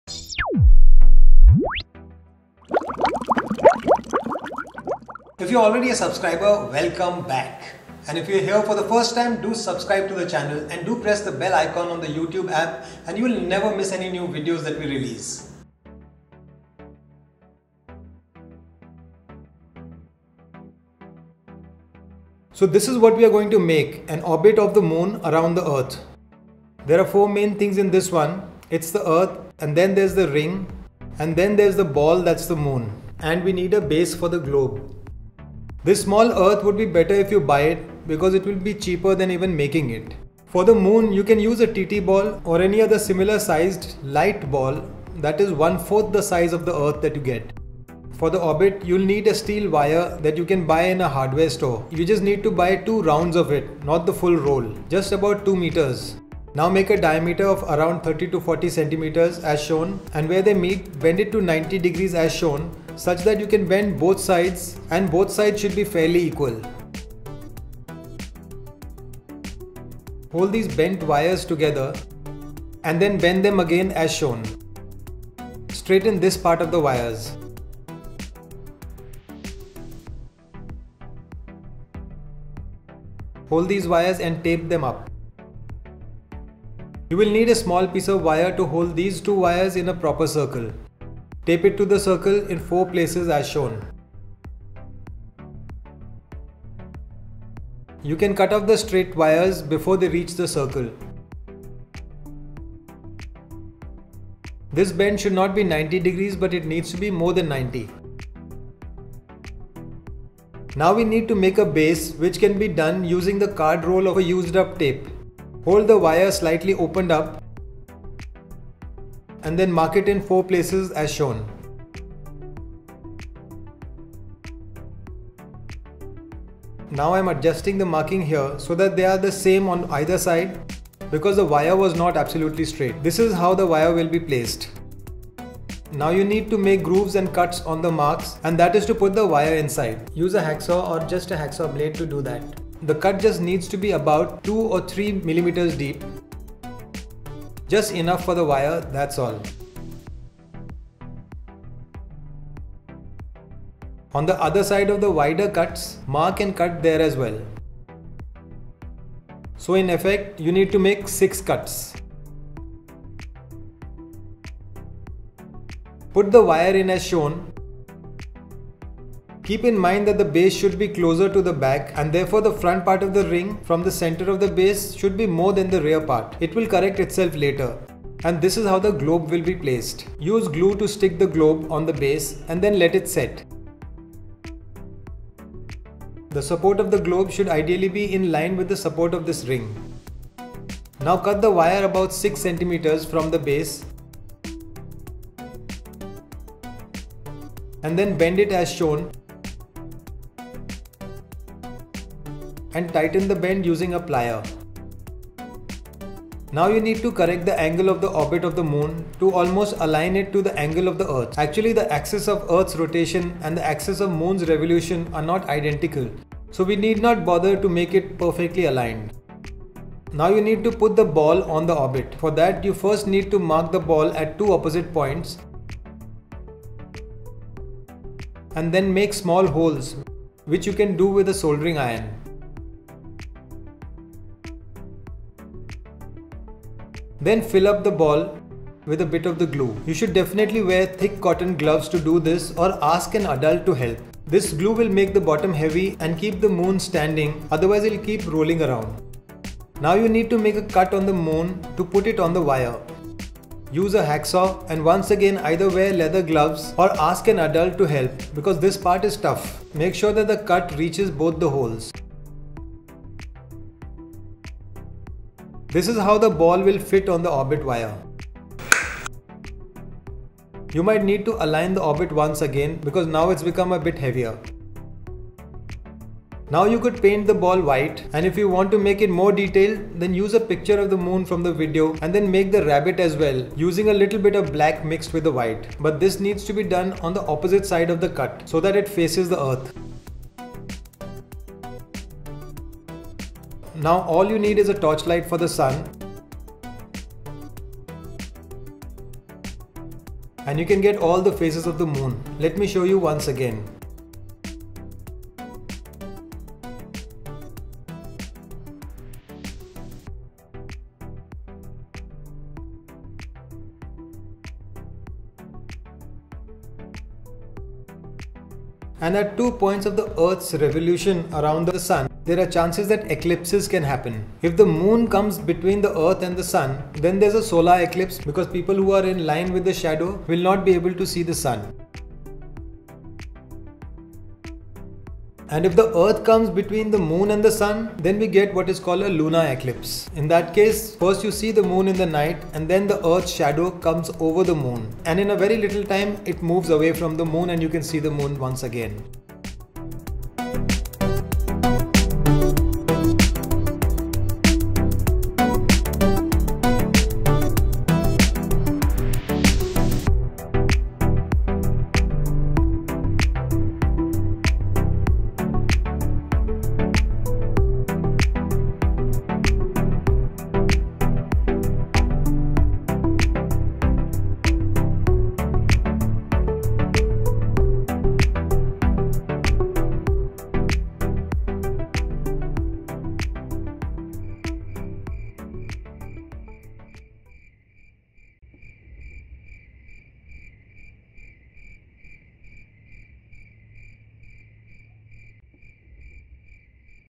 How to show the Phases of the Moon | dArtofScience
This video will show you how to easily make a D.I.Y project that shows how the light from the Sun reflects from the Moon and how it creates different shadows that cause what is called as the different 'Phases of the Moon'. It's an interesting and fun D.I.Y which you can make as a school project as well.